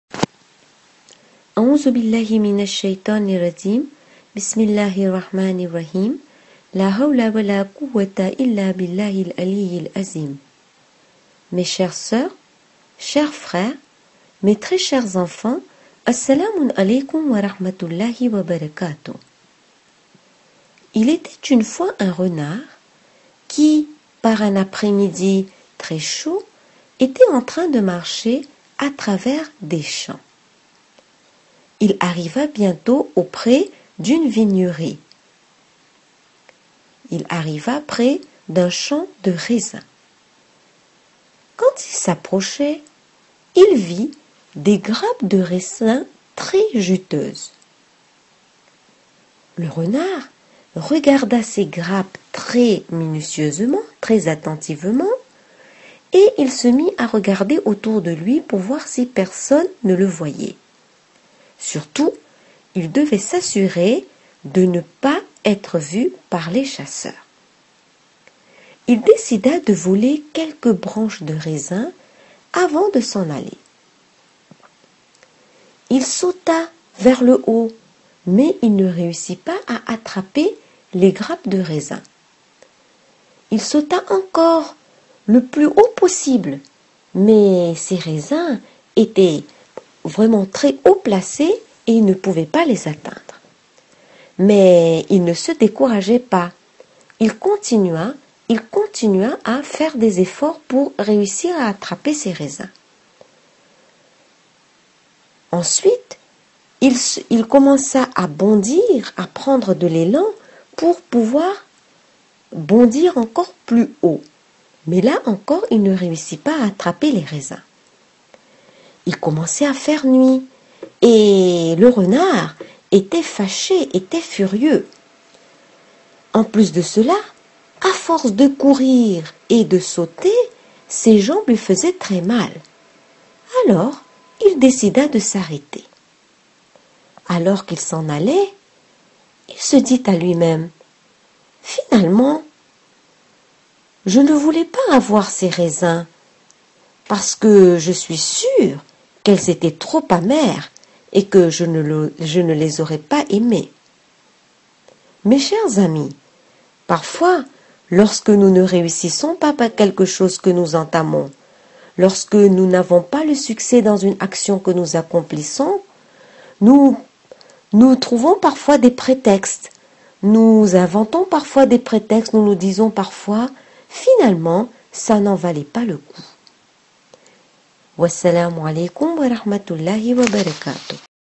histoire morale le renard et les raisins
l'histoire d'un renard qui voulait voler des raisins dans un vignoble...